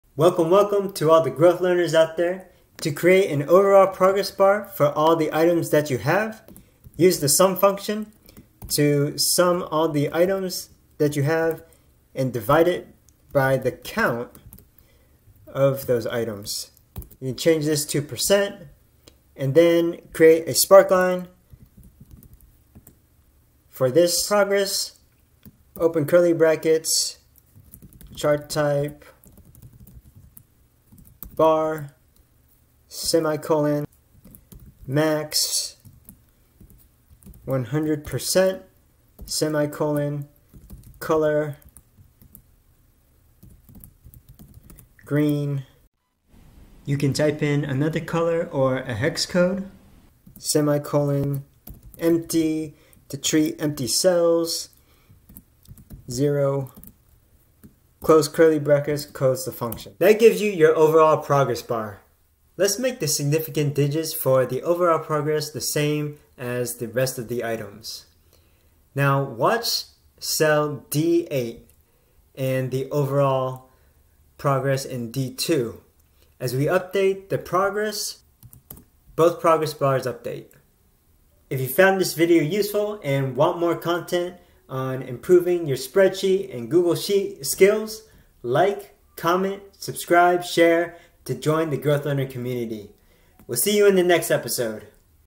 Creating Overall Progress Bar in Google Sheets
To create an overall progress bar in Google Sheets, we can use the sum function divided by the count function, along with the sparkline function. This allows us to visualize the progress data in a single bar, making it easier to track the overall progress.

Use the SUM function to calculate the sum of all the progress values in the specified range. The SUM function adds all the numbers in a range of cells.

Here's an example of how you can use the SUM function to calculate the sum of the progress values in a range:

=SUM(A1:A10)

In this example, the range A1:A10 contains the progress values. The SUM function will return the sum of all the numbers in this range.

To count the total number of progress values in the specified range in Google Sheets, you can use the COUNT function. The COUNT function counts the number of cells in a range that contain numbers or dates.

Here's an example of how you can use the COUNT function to count the progress values in a range:

=COUNT(A1:A10)

In this example, the range A1:A10 contains the progress values. The COUNT function will return the total count of cells in this range that contain numbers.

To create an overall progress bar from multiple progress values, you can divide the sum of the progress values by the count of progress values.

Here's an example of how you can calculate the overall progress using the sum and count functions:

=SUM(A1:A10)/COUNT(A1:A10)

In this example, the range A1:A10 contains the progress values. The SUM function calculates the sum of all the progress values, and the COUNT function calculates the total count of progress values. The result of the division gives you the overall progress.

Lastly, if you want to visualize the overall progress using a sparkline, you can use the SPARKLINE function. The SPARKLINE function creates a miniature chart to represent data in a single cell.

Here's an example of how you can use the SPARKLINE function to create a sparkline for the overall progress:

=SPARKLINE([overall progress data],{"charttype","bar";"max",100%;"color1","[color]";"empty","zero"})

The SPARKLINE function takes the result of the division (overall progress data) as its argument. It will create a sparkline chart in a single cell to represent the overall progress visually.